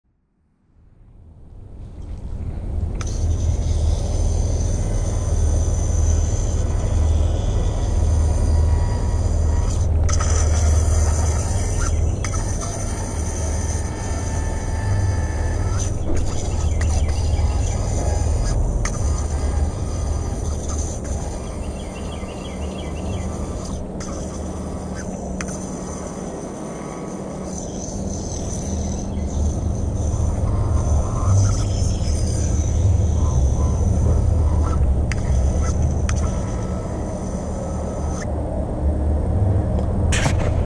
Devil Wire Walker with bearing spintop
Can a bearing top do a wire walker? Or is it an illusion?
Trick named by Jolien Tomme